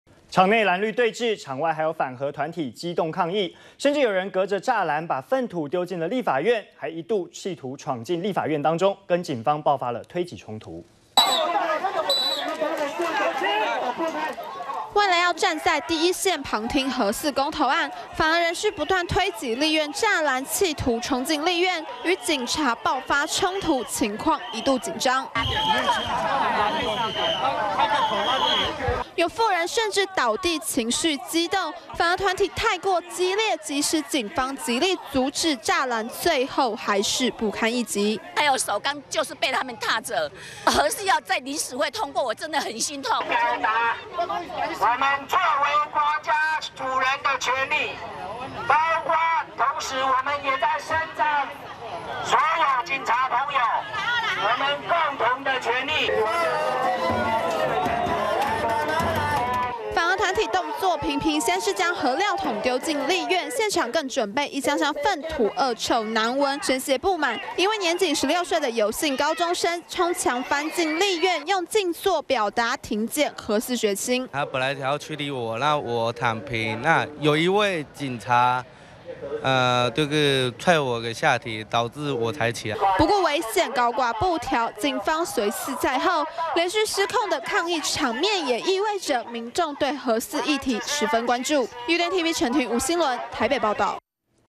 【2013.08.02】反核民眾立院抗議 丟糞土扯柵欄 -udn tv
場內藍綠對峙，場外還有反核團體激動抗議，還有人隔著柵欄，把糞土丟進立法院，甚至一度企圖闖進立法院，和警方爆發推擠衝突。udn tv官網 , udn tv on FB , Twitter , G+